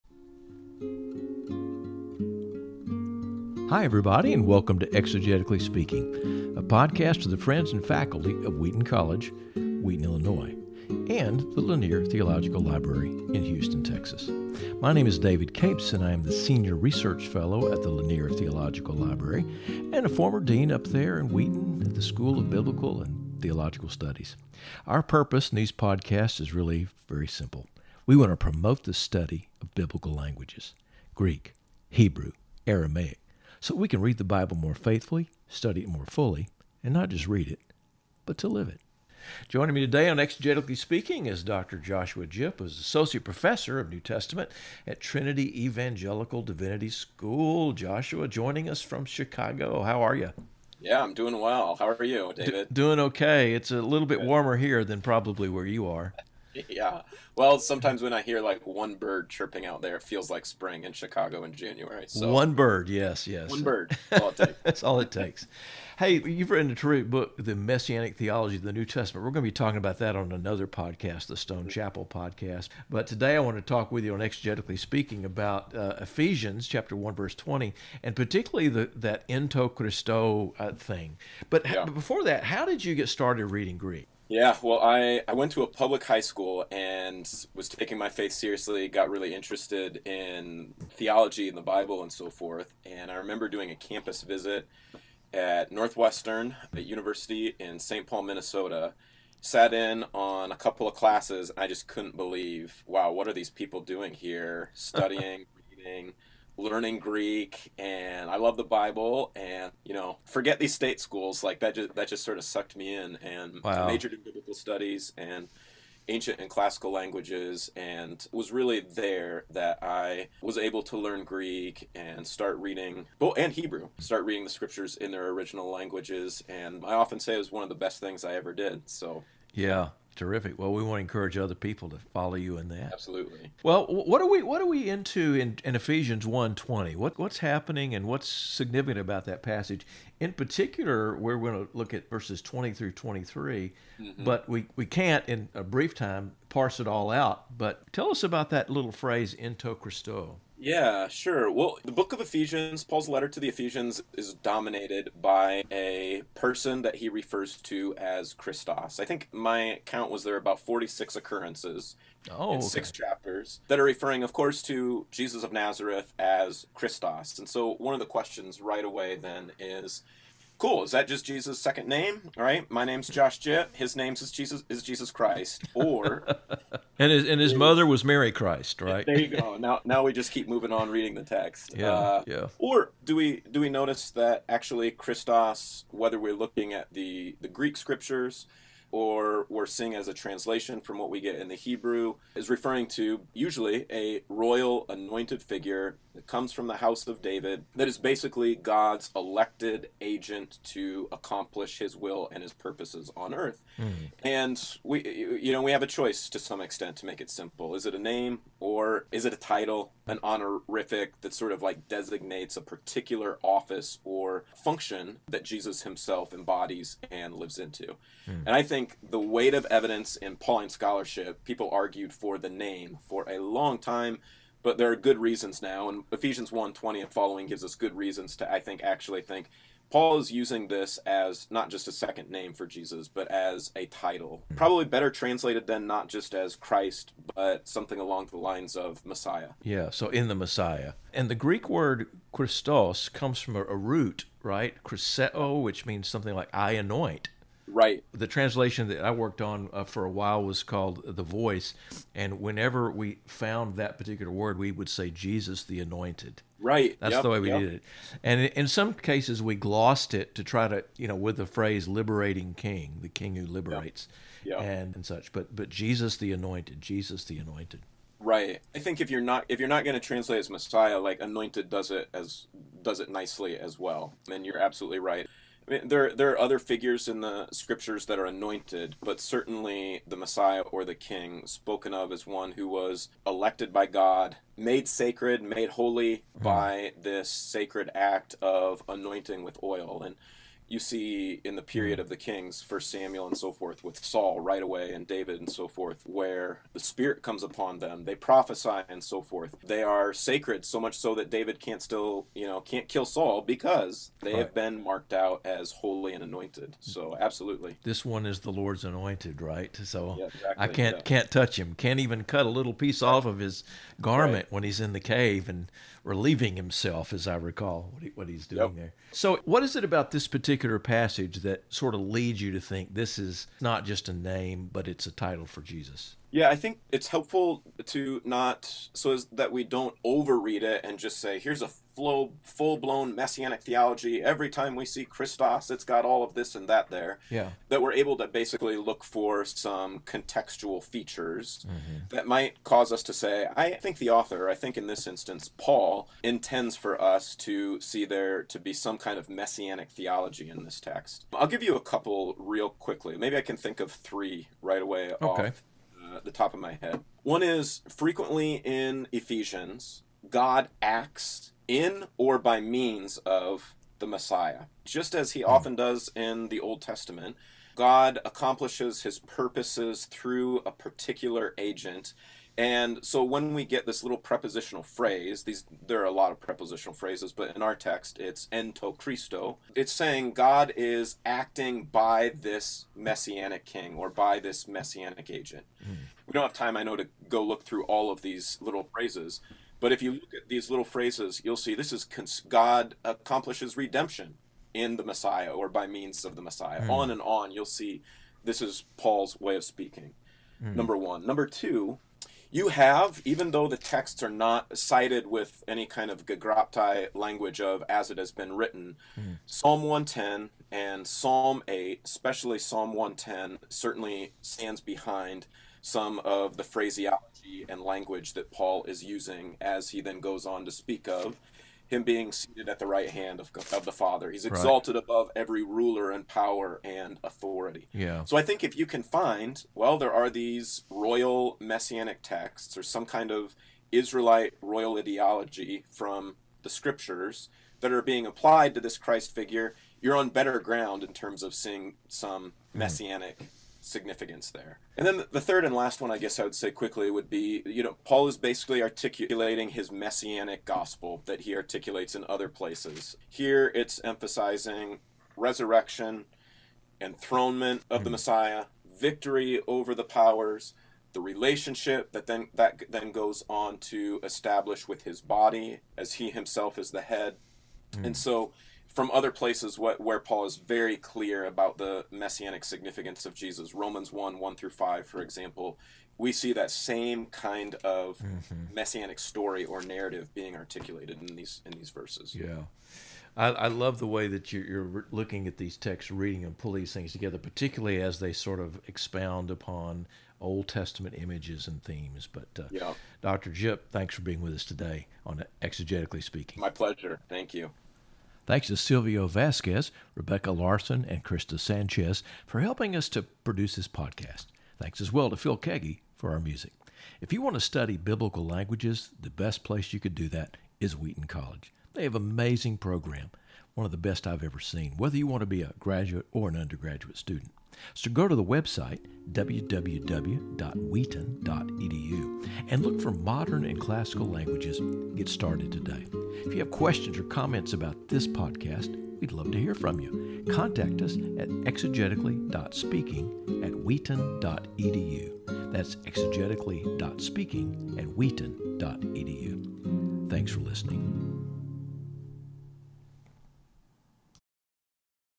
In Christ: Eph. 1:20-23 - with Dr. Joshua Jipp
Dr. Joshua Jipp is Associate Professor of New Testament at Trinity Evangelical Divinity School. Among his other publication are Saved by Faith and Hospitality (Eerdmans, 2017) and The Messianic Theology of the New Testament (Eerdmans, 2020). He argues that “Christ” is not only a name for Jesus when it is used in many NT passages, but a title: messiah, anointed one. He talks about the OT background and significance of this title when it is used by Paul in Ephesians and elsewhere.